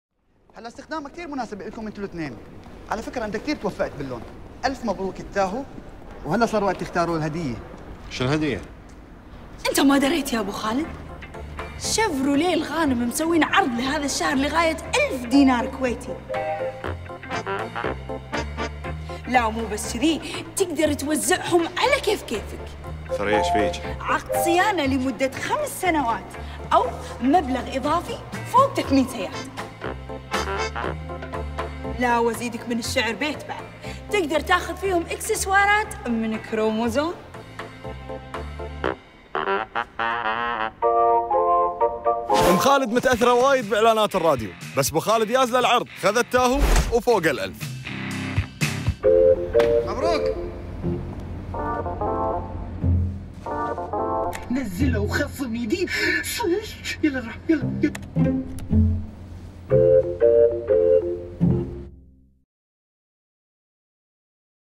عروض شفرولية في فبراير 2017
هديتك لغاية 1000 دينار عند شرائك شفرولية خلال شهر فبراير توزعها على كيفك بين عقد صيانة، مبلغ إضافي فوق التثمين أو اكسسوارات من كروموزون!